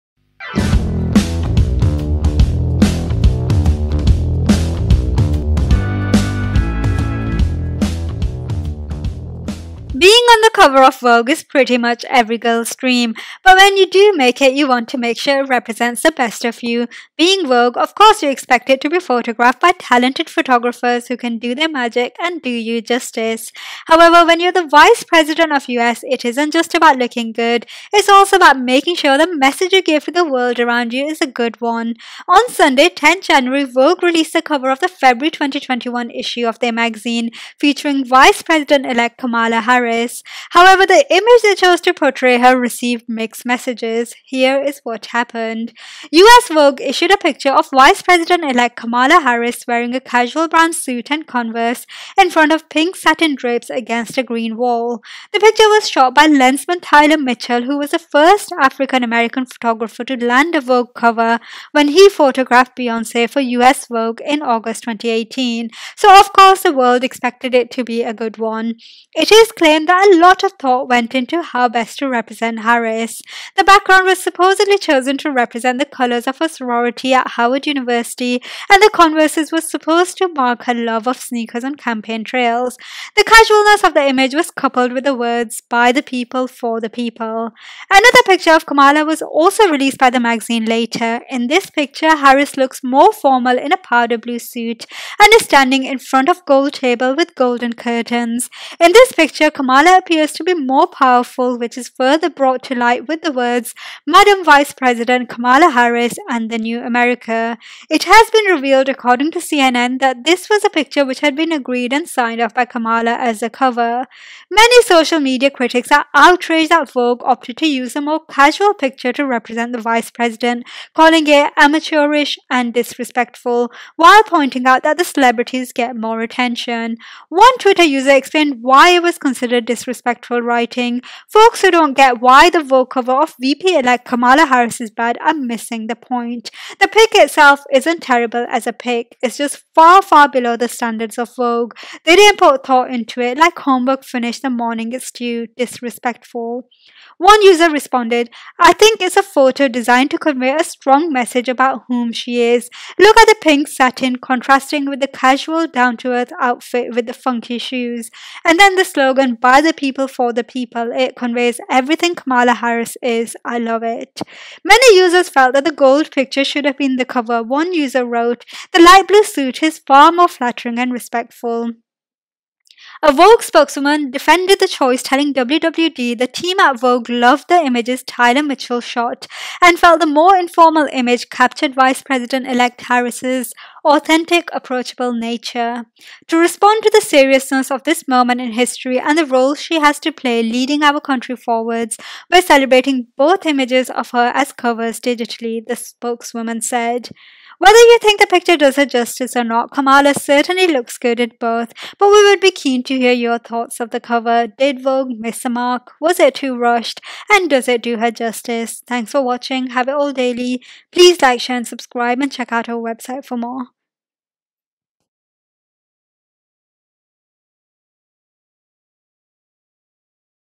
Why is Kamala Harris' Vogue Cover Causing Controversy?
TItle: Why is Kamala Harris' Vogue Cover Causing Controversy?

Description:Being on the cover of Vogue is pretty much every girl’s dream but when you do make it you want to make sure it represents the best of you. Being vogue of course you expect to be photographed by talented photographers who can do their magic and do you justice.

However when you are vice-president of US, it isn’t just about looking good, it’s also about making sure the message you give to the world around you is a good one. On Sunday 10 January Vogue released the cover of the February 2021 issue of their magazine featuring vice-president elect Kamala Harris. However the image they chose to portray her received mixed messages. Here is what happened.

US Vogue issued a picture of vice-president elect Kamala Harris wearing a casual brown suit and converse in front of pink satin drapes against a green wall. The picture was shot by Lensman Tyler Mitchell, who was the first African American photographer to land a Vogue cover when he photographed Beyoncé for US Vogue in August 2018. So of course the world expected it to be a good one.

It is claimed that a lot of thought went into how best to represent Harris. The background was supposedly chosen to represent the colours of her sorority at Howard University and the converses were supposed to mark her love of sneakers on campaign trails. The casualness of the image was coupled  with the words “By the People, for the people”

Another picture of Kamala was also released by the magazine later. In this picture Harris looks more formal in a powder blue suit and is standing in front of a gold table with golden curtains. In this picture Kamala appears to be more powerful which is further brought to light with the words ‘Madam Vice President! Kamala Harris And The New America” It has been revealed according to CNN that this was the picture which had been agreed and signed off by Kamala as the cover.

Many social media critics are outraged that Vogue opted to use the more casual picture to represent the vice president calling it “amateurish” and “disrespectful” while pointing out that the celebrities get more attention.

One twitter user explained why it was considered disrespectful writing; “Folks who don't get why the Vogue cover of VP-elect Kamala Harris is bad are missing the point,” “The pic itself isn't terrible as a pic. It's just far, far below the standards of Vogue. They didn't put thought into it. Like homework finished the morning it's due. Disrespectful.”

One user responded’

I think it's a photo designed to convey a strong message about whom she is. Look at the pink satin contrasting with the casual, down to earth outfit with the funky shoes. And then the slogan: "By the people, for the people". It conveys everything
is. I love it.

Many users felt that the gold picture should have been the cover. One user wrote; “The light blue suit is far more flattering and respectful.

A Vogue spokeswoman defended the choice telling WWD, “The team at Vogue loved the images Tyler Mitchell shot and felt the more informal image captured Vice President-elect Harris’ authentic, approachable nature...

 To respond to the seriousness of this moment in history, and the role she has to play leading our country forwards, we’re celebrating both images of her as covers digitally.”  The spokeswoman said.

Whether you think the picture does her justice or not, Kamala certainly looks good in both, but we would be keen to hear your thoughts of the cover. Did Vogue miss the mark? Was it too rushed and does it do her justice?